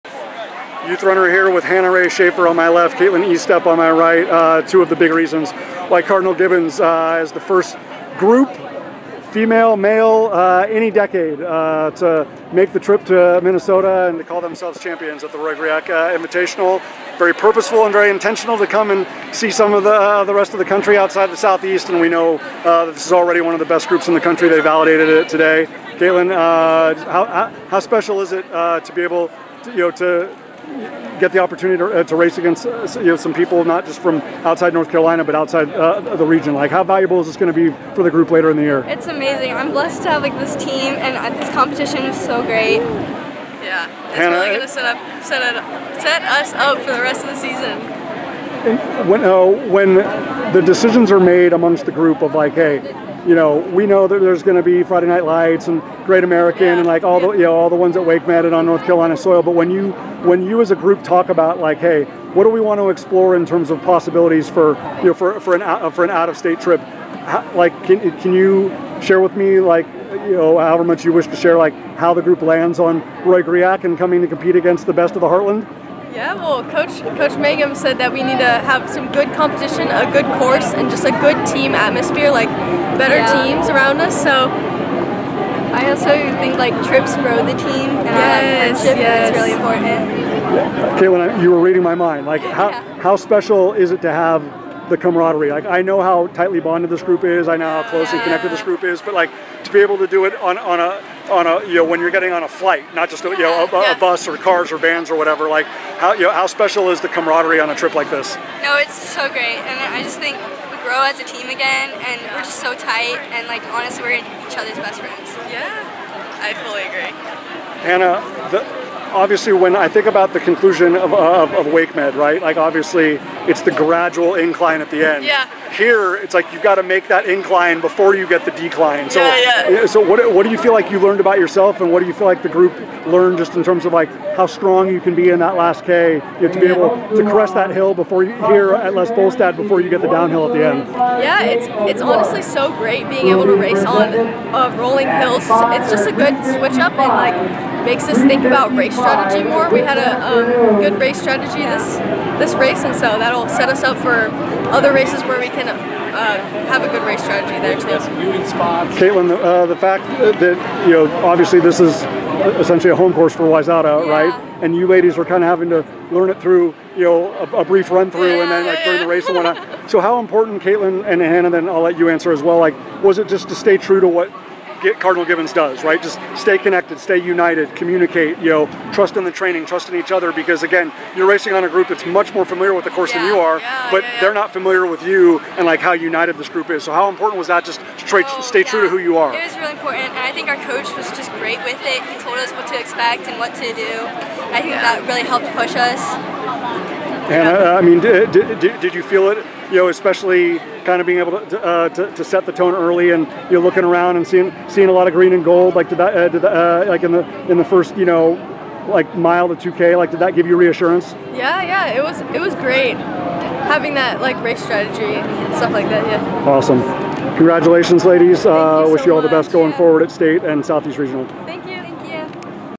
Hannah Rae Shaffer Takes the Lead at Roy Griak 5K Championship
Cardinal Gibbons teammates Hannah Rae Shaffer, a senior, and freshman Kaitlyn Estep finished sixth and ninth, respectively, in the girls 5-kilometer championship race at the 38th Roy Griak Invitational at Les Bolstad Golf Course in Minnesota. Shaffer clocked 18:48.2 and Estep ran 18:55.1 for the Crusaders, who became the first North Carolina program in meet history to win a team title, following a 94-point performance.